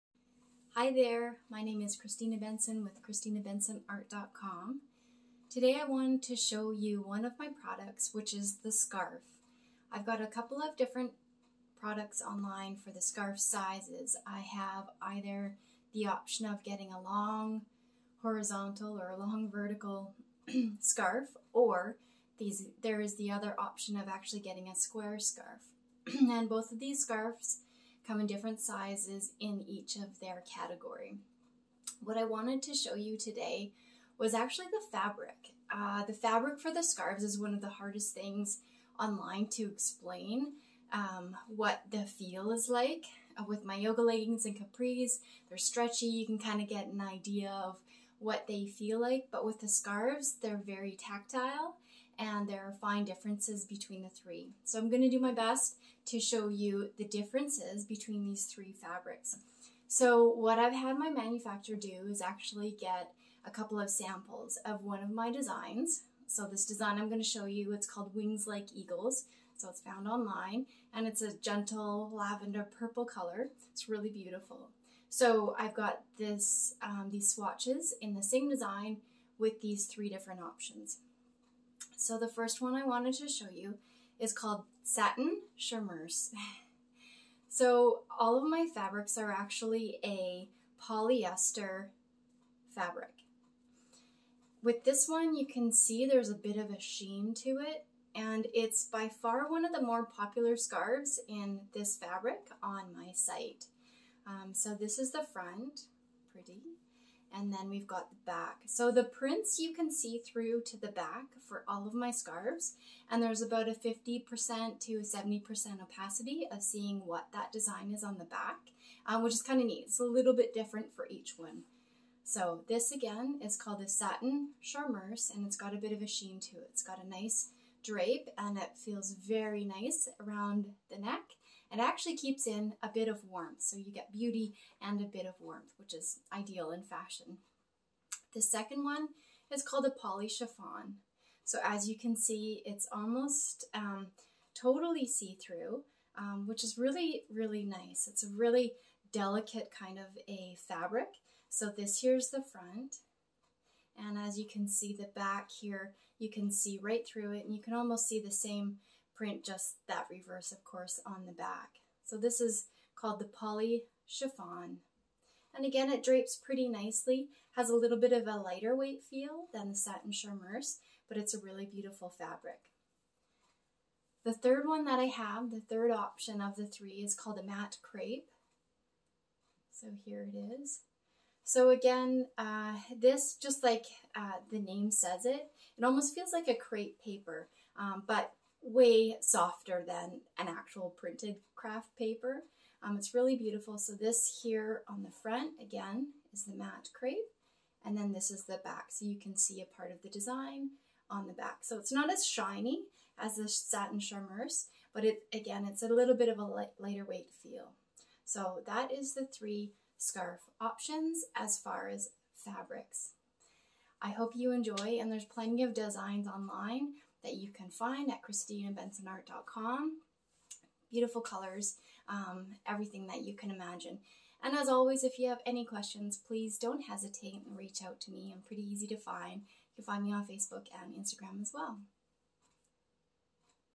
Scarves - 3 Different Fabric Options
What is the difference between the three scarf fabrics?

Scarves

Square or Vertical options to choose from

Fluid and beautiful draped scarves are the perfect way to wear art wherever you go.

Poly Chiffon: 100% poly, light weight, very soft, lovely drape, elegant and see through
Satin Charmeuse: 100% poly satin weave, mimics silk, lovely drape, nice sheen, opaque (Our bestseller!)
Matte Crepe: 100% polyester fancy weave, soft 'crepe paper' feel, slightly see through

These gorgeous and unique scarves add a pop of colour to any outfit. My wearable art scarves make great gifts for that hard-to-shop-for person in your life.

Unique Wearable Art by Kristina Benson
Made in Canada